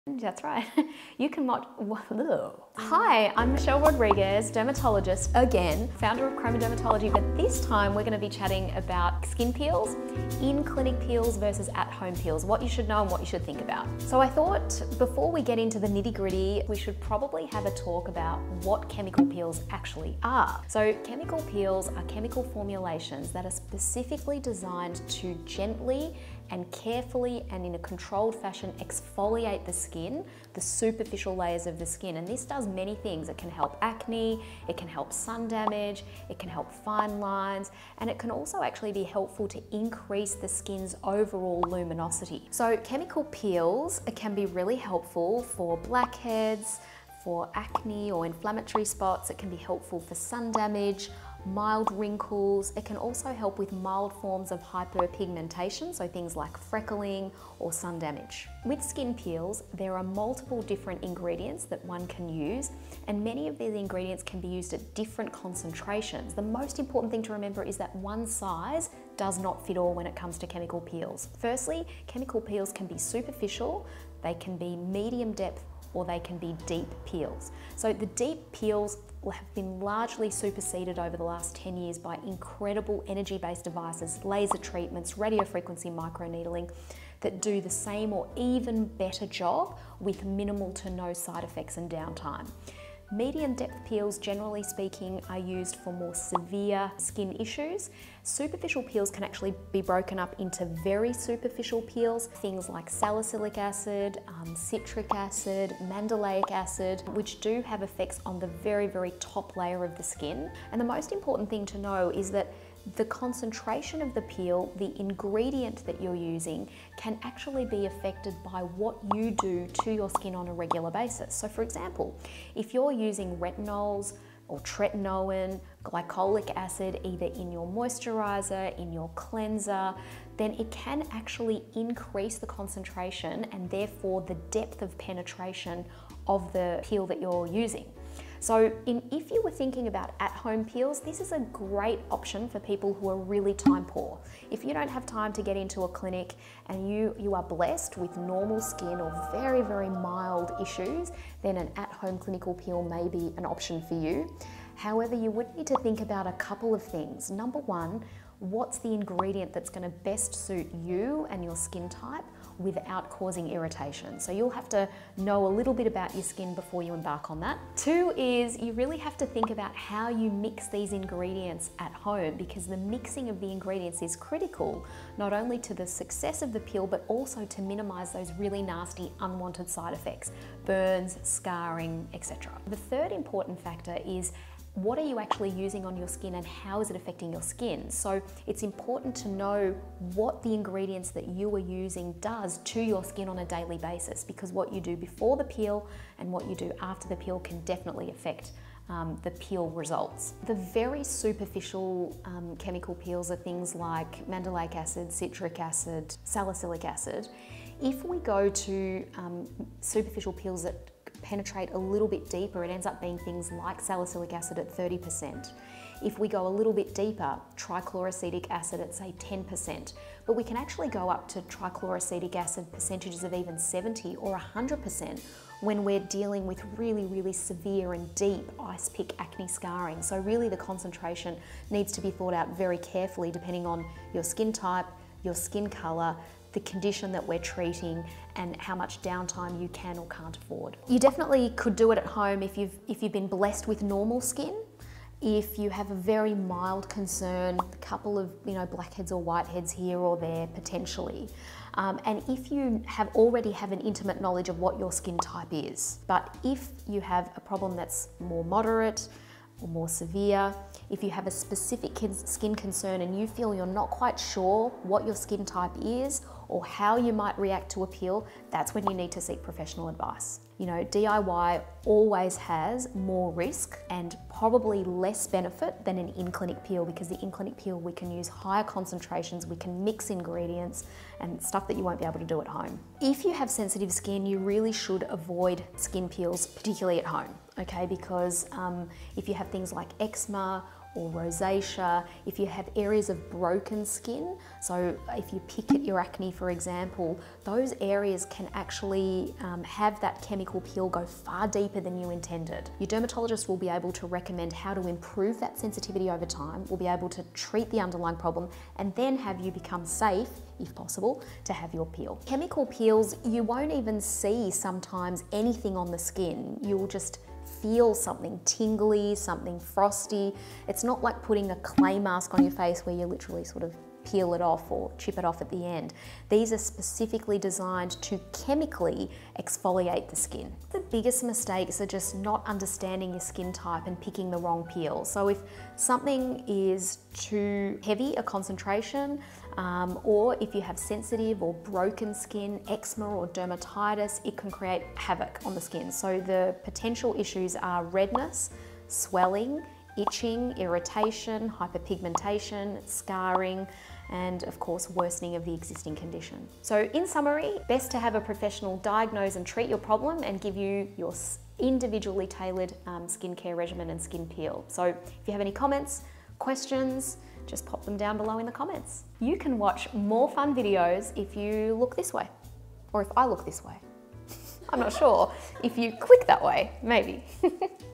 Chemical Peels At Home Vs Professional Chemical Peels | Chemical Peel Benefits
Dr Michelle Rodrigues explains the benefits of chemical peels, the different kinds of peels and when you should (and shouldn't!) do a peel at home.

00:00 What is a chemical peel?
00:45 Chemical peel benefits
01:13 Different types of peels
02:20 When to use an at home peel
04:43 Peels for sensitive skin
05:32 Biggest peel mistakes